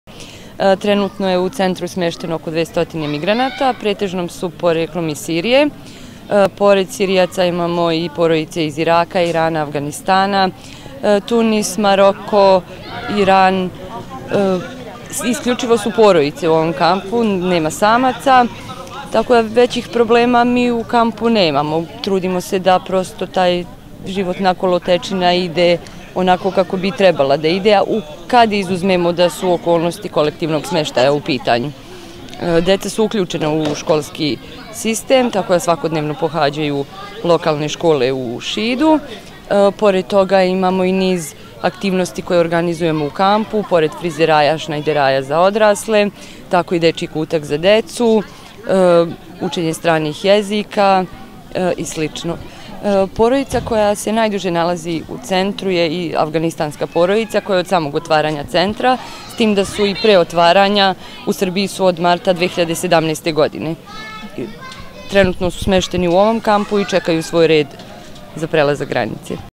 Tanja Marić - Migranti
Danas je Međunarodni dan migranata koji se obeležava  po odluci Ujedinjenih Nacija uz podatak da se sada  u svetu  nalazi 215 miliona stanovnika koji imaju ststus migranta ili izbeglice. Trenutno je u 17 prihvatnih i centara za azil kojima upravlja komesarijat smešteno 4427 migranata, od kojih 807 dece. U tri prihvatna  centra na području šidske opštine trenutno boravi oko 1400 migranata